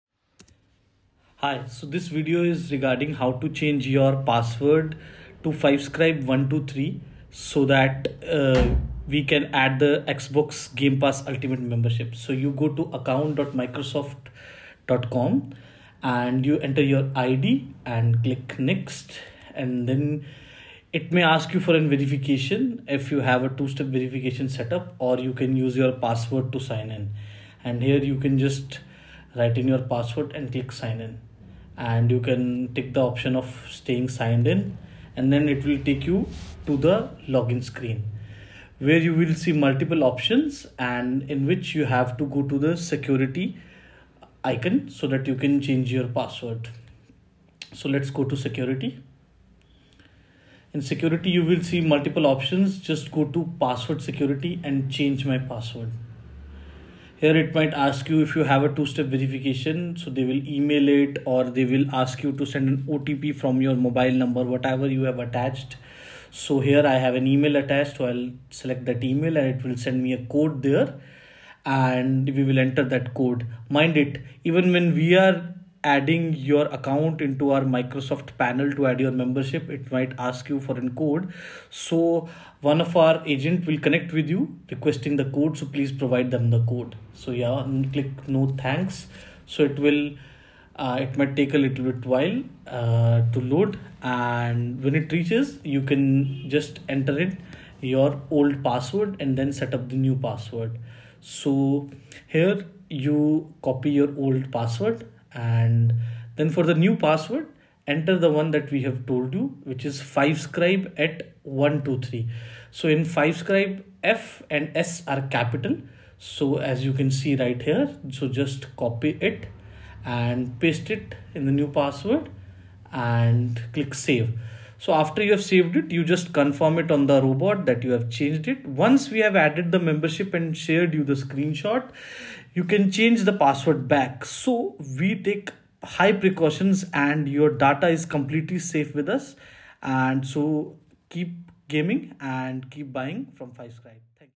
How to Change Your Microsoft Account Password to Add Game Pass Membership by FiveScribe
In this video, we will show you how to change your Microsoft account password in order to add a membership to your account. Whether you want to sign up for Xbox Game Pass, Office 365, or any other Microsoft service, you'll need to have a valid Microsoft account with an up-to-date password.

To get started, you'll need to log in to your Microsoft account using your current password. Once you're logged in, navigate to the "Security" section of your account settings, where you'll find the option to change your password.

In the password change screen, you'll need to enter your current password and then create a new one. Make sure your new password is strong and unique, with a mix of uppercase and lowercase letters, numbers, and special characters. Avoid using common words or phrases that can be easily guessed.

Once you've updated your password, you can go ahead and add your desired membership to your account. Simply navigate to the service you want to sign up for, and follow the prompts to complete the sign-up process.

Remember to keep your password secure and change it regularly to help protect your Microsoft account and personal information. And that's it! Now you know how to change your Microsoft account password and add a membership to your account.